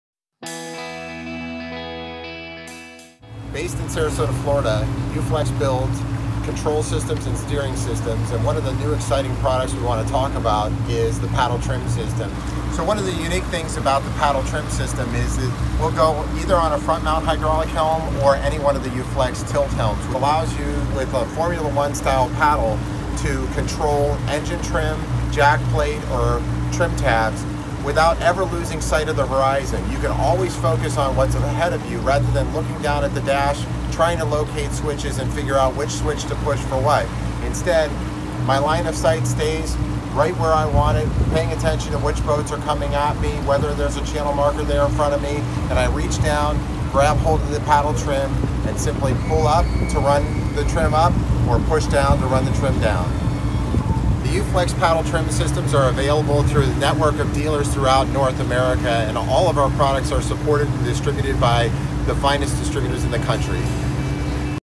Paddle Trim - Easy control of your boat by Uflex steering
For complete control of engine trim and tilt, trim tabs, jackplates, PowerPole(R) and other on board systems, the innovative Paddle Trim system is the answer.

Adaptable to existing front mount hydraulic and tilt helms, the Paddle Trim is ergonomically designed to allow the driver to make precise adjustments to the attitude of the boat or other systems without removing his hand from the steering wheel.

FEATURES

• Available both in single and dual versions (for either two equal or two different outputs).

• Both single or dual paddle can be positioned at various angles for the most confortable choice.

• Equipped with one water proof “MOM.ON-OFF-MOM.ON” switch (2 switches on dual version).

• Max load 2 Amp @ 12VDC.

• Kit relay available KTR - 42860S:

- It allows a Paddle Trim pair to control electrical loads up to 10 Amp (e.g. Trim Tabs Uflex®).

- Easy to use and to install, through connectors with pre-crimped contacts.

• Suitable for any kind of steering wheel dish. Max wheel dia. 400 mm (15.7”).

• Easy to install on the following Ultraflex® steering systems:

- front and tilt mount hydraulic helms

- tilt mount mechanical helms

• Wiring harness, labels and mounting hardware kit are included. The mounting plate, which differs between front and tilt mount installation, is to be ordered separately.